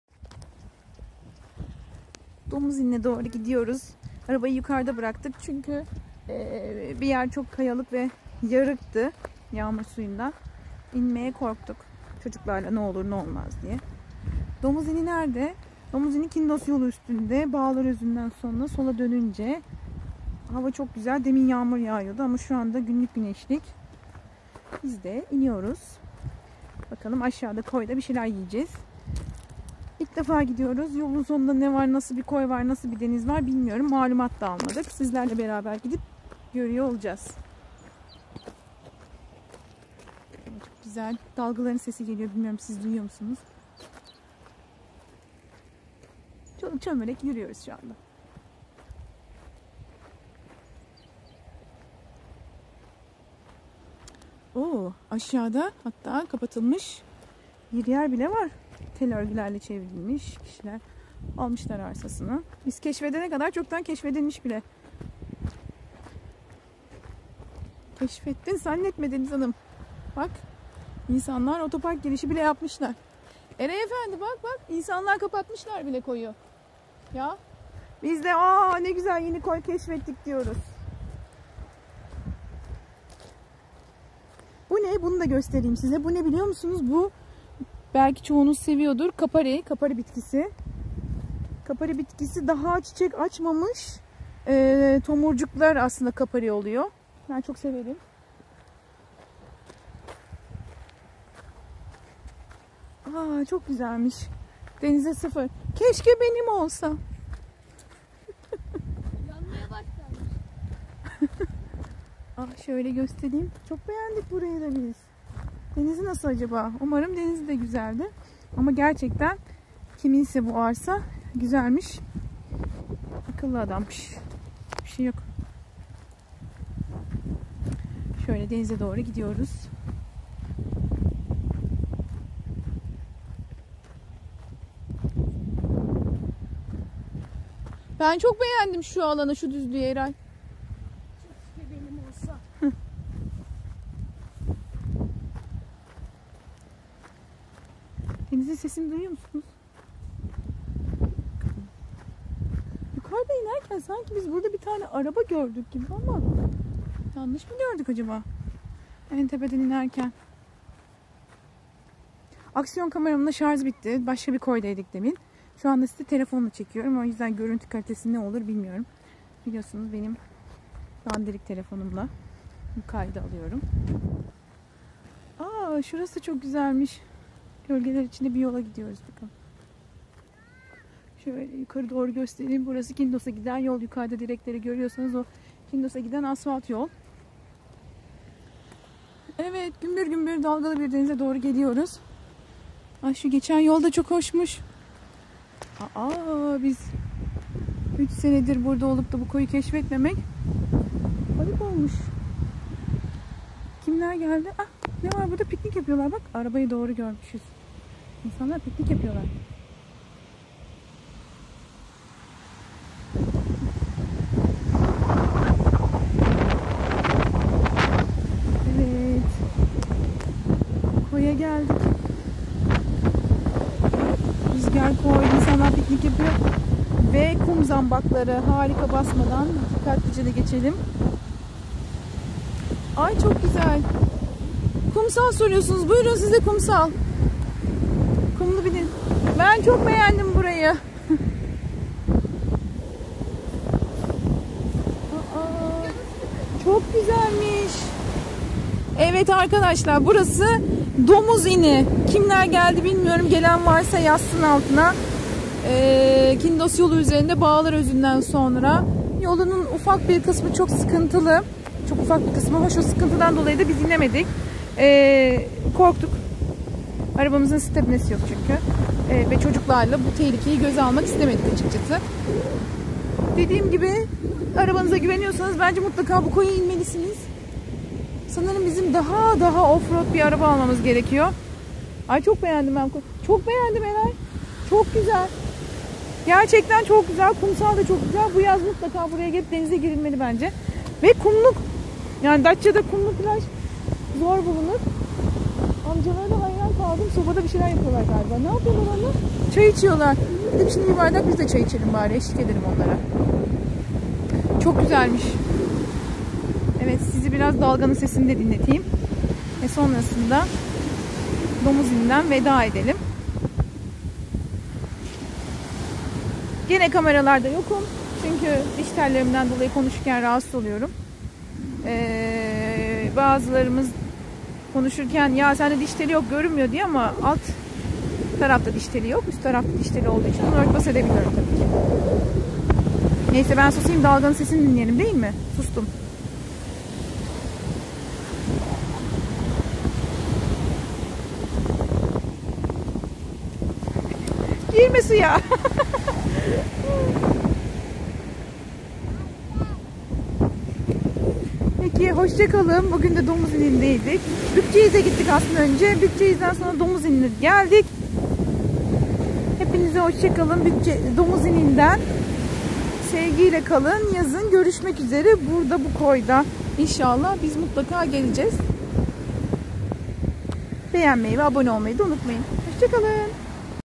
Datça - Domuzini
Merhaba Arkadaşlar,

Bu sefer de Datça Domuzini koyundan size harika görüntüler gönderiyoruz. Ben de ilk defa gittim. Hep beraber bu güzel koyu gezmeye ne dersiniz...